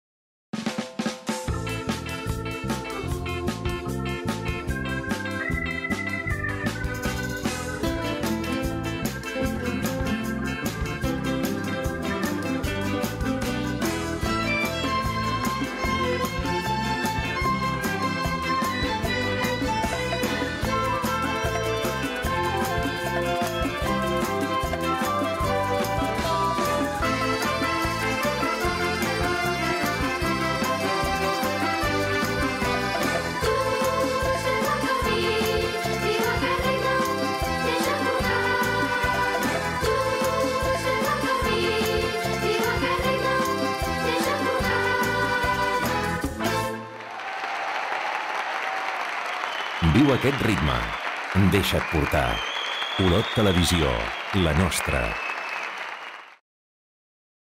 Espot promocional Escola de Música i Olot Televisió 2013
Espot promocional de l'escola de Música i Olot Televisió emès l'any 2013.

La música va ser ideada pels alumnes de composició de l'escola amb l'assesorament i arranjament dels professors de l'assignatura.

Aquesta música va ser gravada per alguns alumnes de l'escola. Espot enregistrat al Teatre Principal de la ciutat d'Olot.